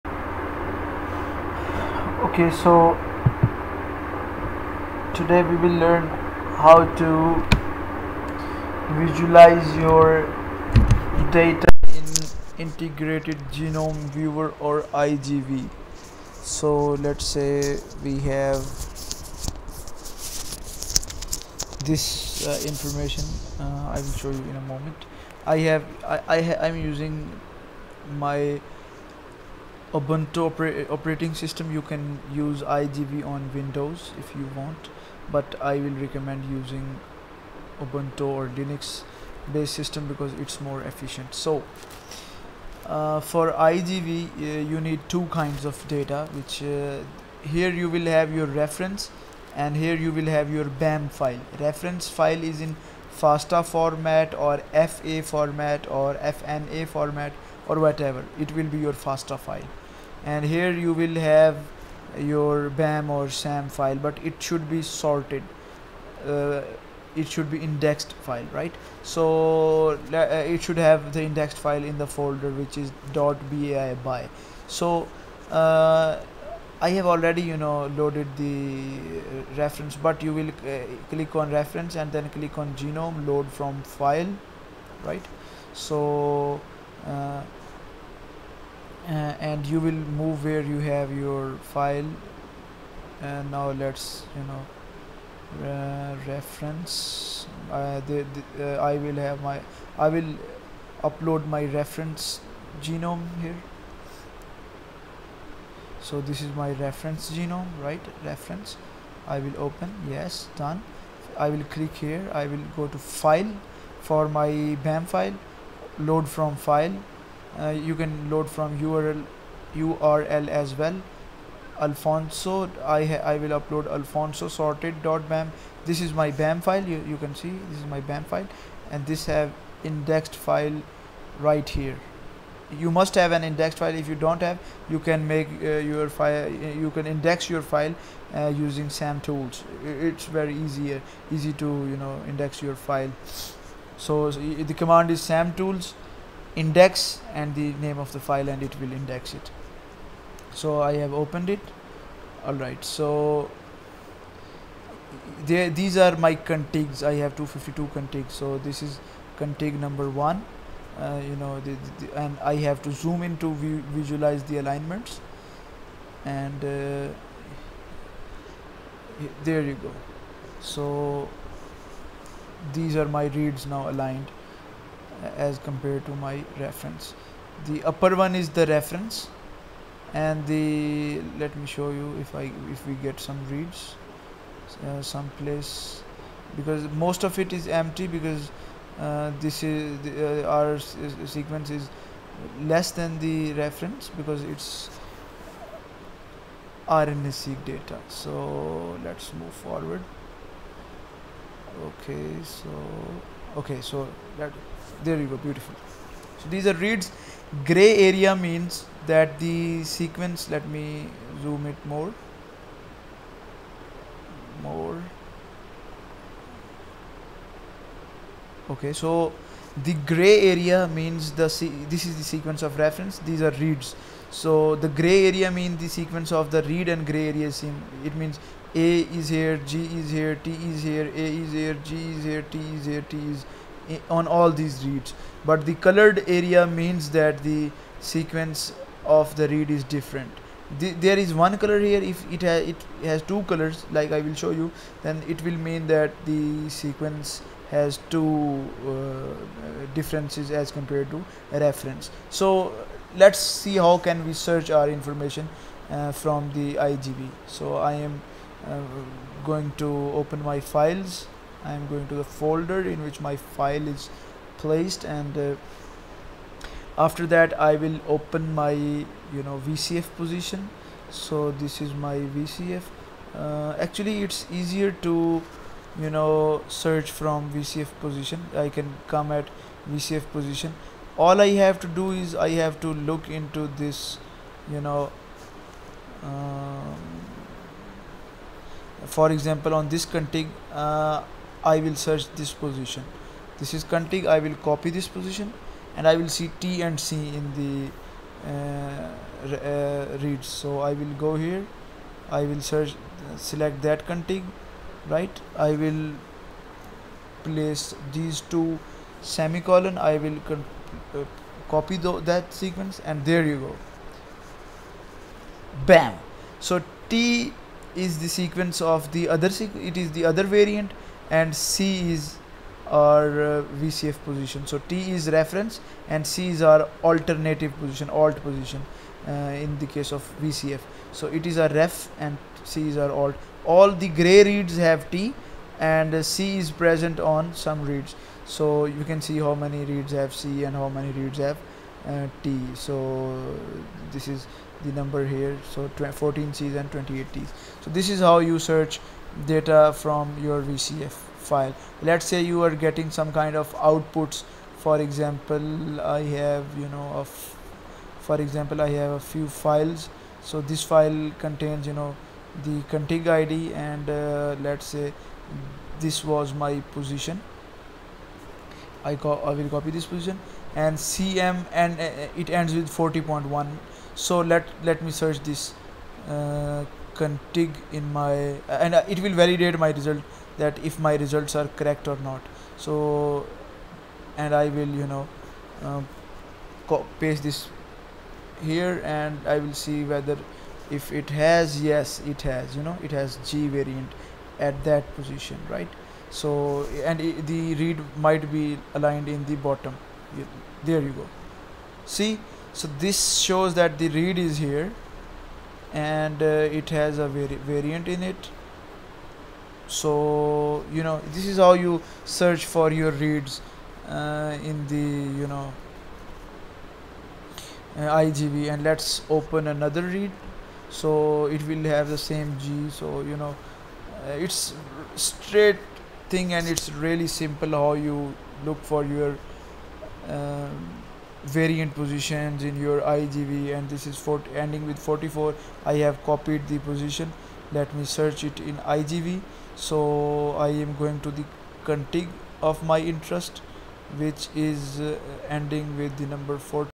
IGV (Integrated Genome viewer) DEMO
How to visualize variants in IGV using variant positions.
IGV is a free open source tool and can be used by anyone. It is used in genomic research widely and after Covid-19 its used for genome analysis has increased drastically.

Special thanks to IGV for the free tool.

Note : This video is only for educational and research purposes and all the tool used are freely available for everyone without any under MIT License 2015.